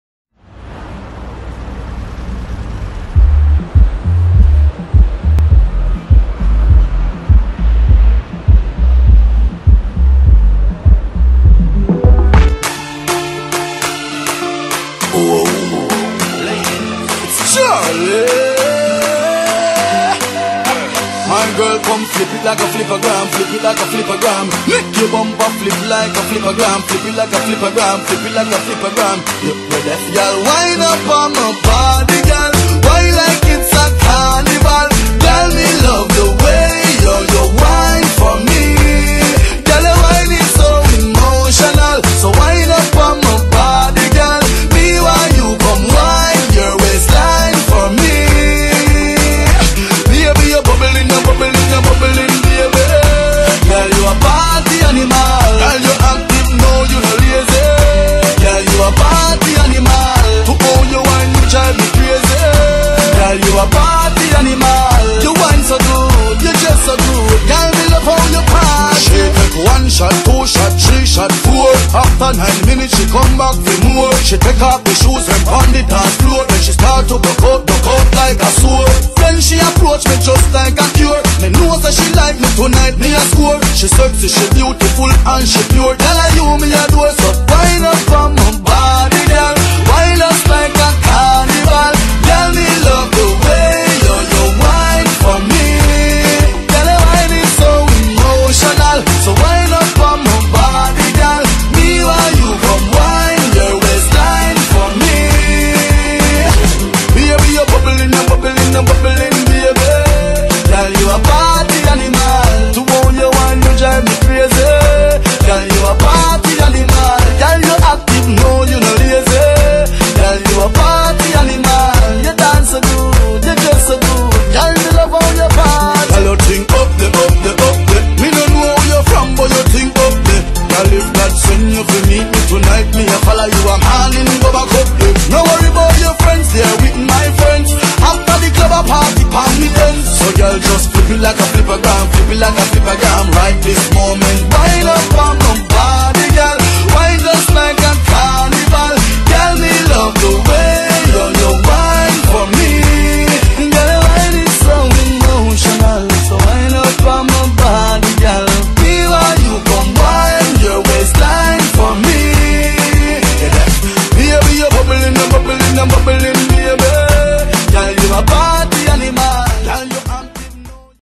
Zumba - Power Gym Osorno
Zumba es un movimiento o disciplina fitness enfocado por una parte a mantener un cuerpo saludable y por otra a desarrollar, fortalecer y dar flexibilidad al cuerpo mediante movimientos de baile combinados con una serie de rutinas aeróbicas se pueden llegar a quemar 800 calorías.